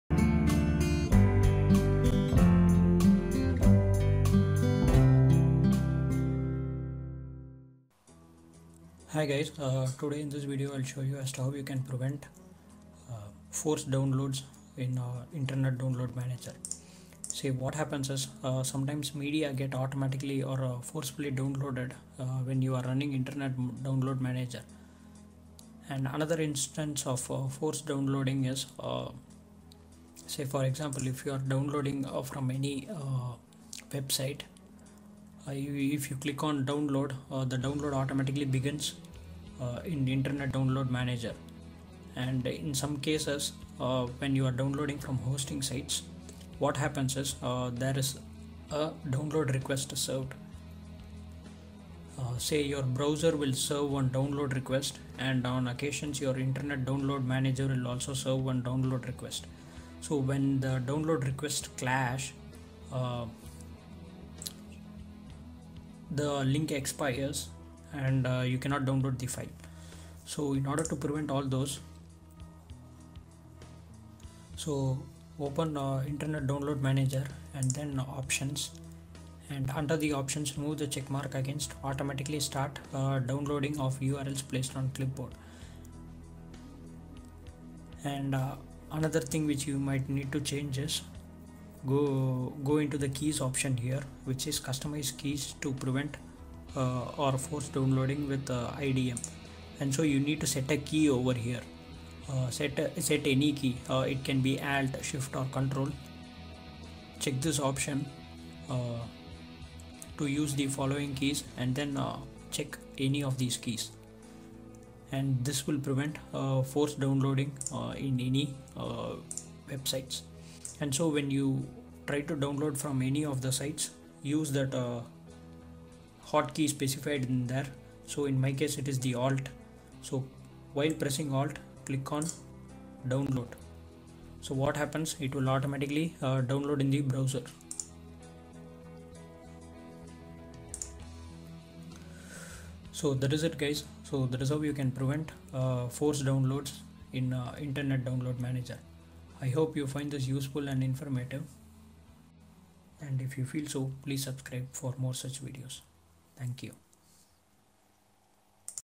Prevent Files From Automatically Downloading In Internet Download Manager
Prevent files from automatically downloading in internet download manager. Choice of downloading using browser even while running IDM. Download using browser even while running Internet download manager.